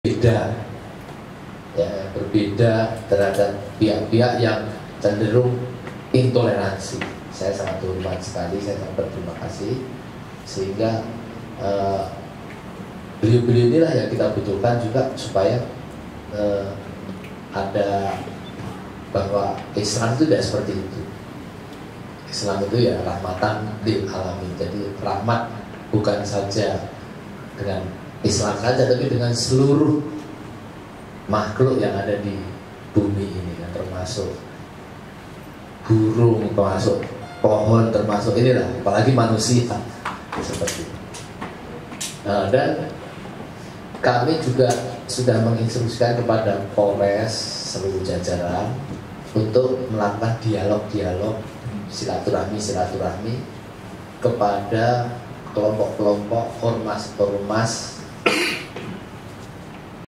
Kapolda Jateng menerima Kunjungan Silaturahmi FKUB Jateng di Loby Mapolda Jateng
Kreatif by agussaibumi